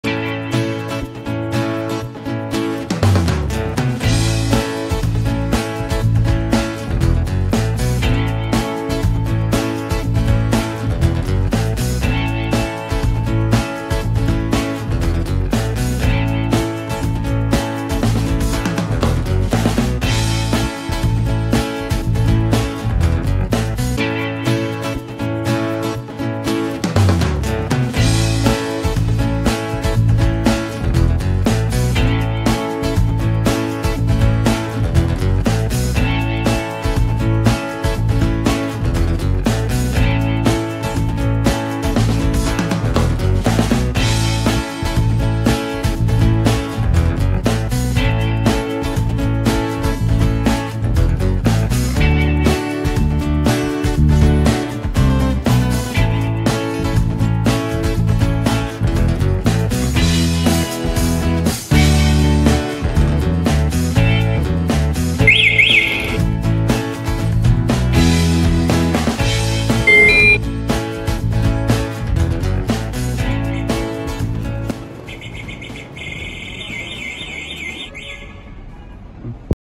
How to assemble the cool EB2 from 7Go (Upgraded version)?
1, how to assemble the front wheel,  the steering wheels, front lamp ?
2, how to assemble the throttle ?
2, how to turn on and turn off the bikes with alarm sensor?